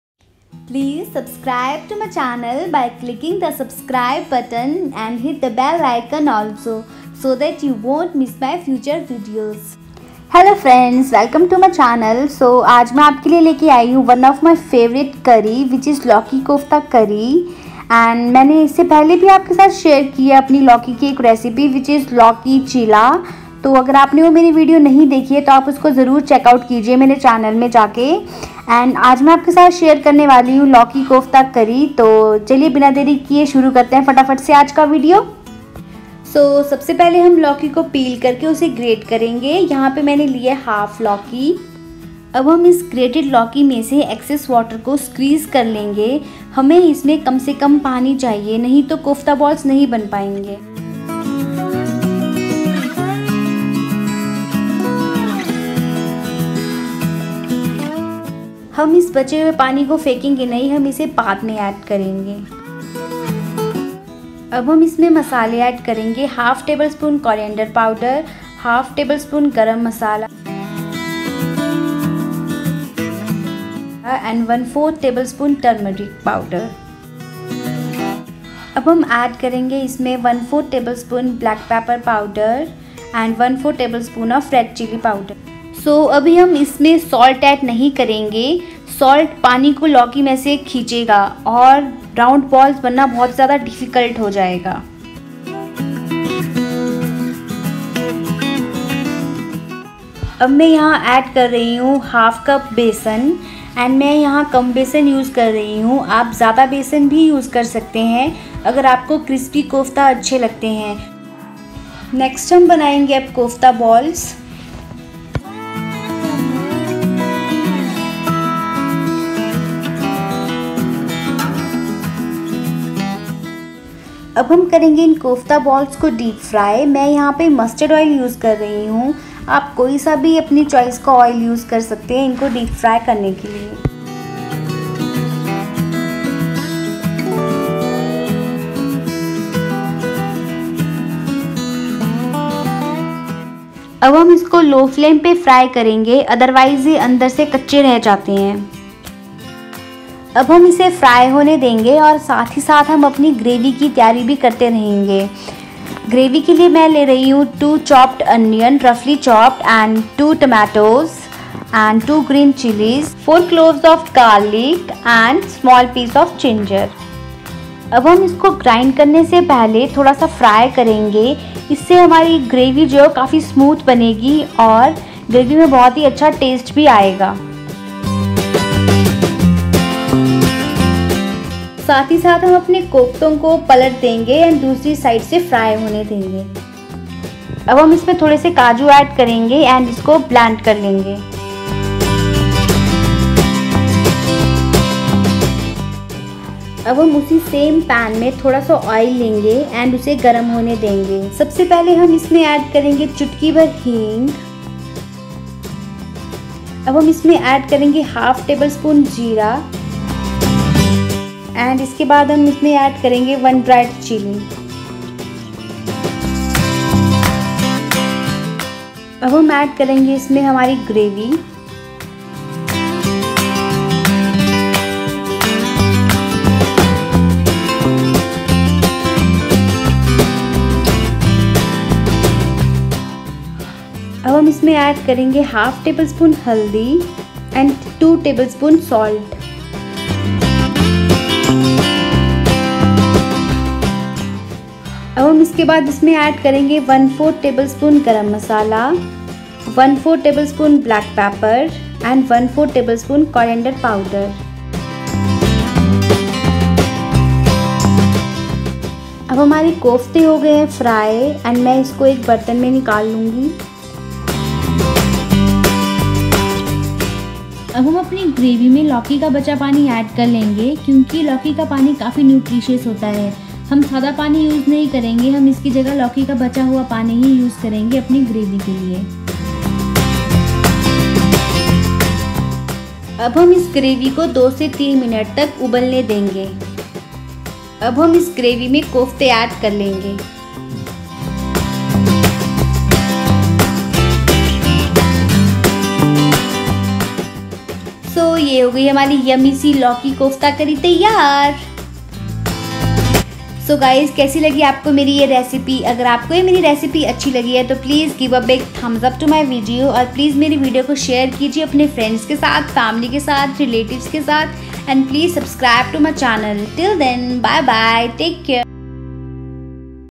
बहुत हीआसान टेस्टी लौकी कोफ्ता रेसिपी | लौकी कोफ्ता करी | Lauki Masala Kofta Curry
Hello Everyone,
                            So today I  wanted to share a very quick and tasty recipe of bottle gourd/Lauki, which is called Lauki Kofta Curry. I hope you will like the recipe and video.